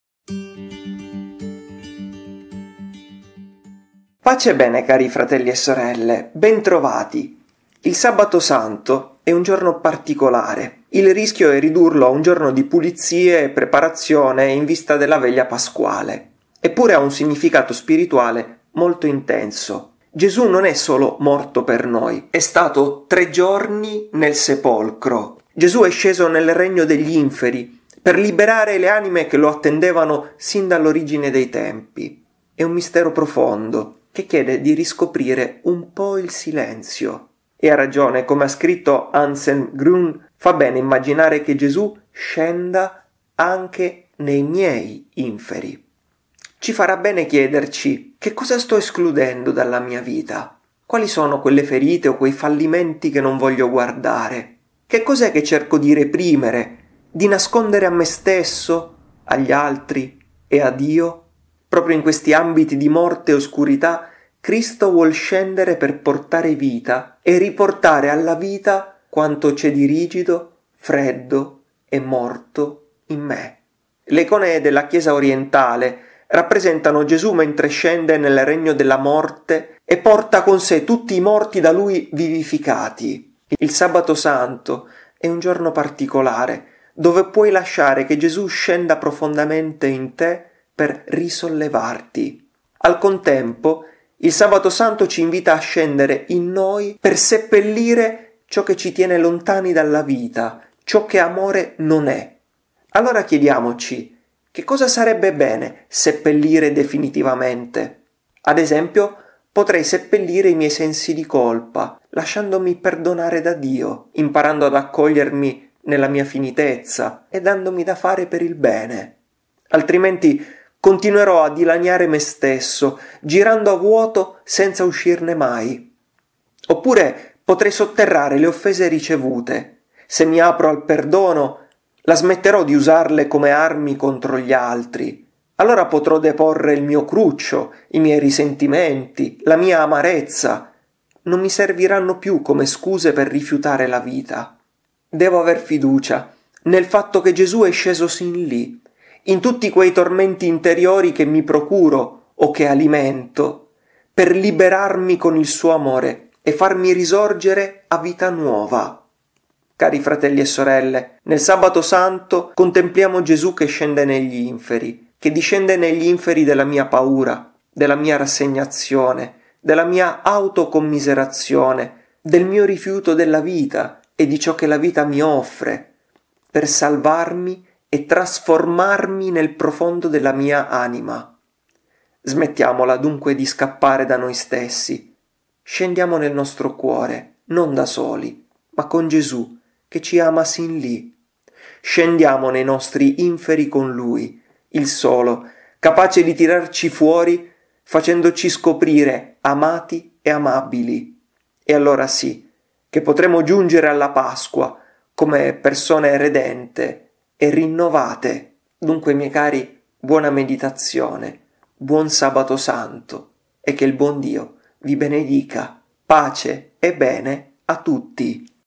Il coraggio di guardarsi dentro, di fra Umile mdv
Scendere e risalire (con Gesù) dai nostri inferi
Piccola meditazione per il Sabato Santo, di fra Umile mdv

Per il commento al Vangelo del giorno scarica l'app "missionari della Via" o vai al sito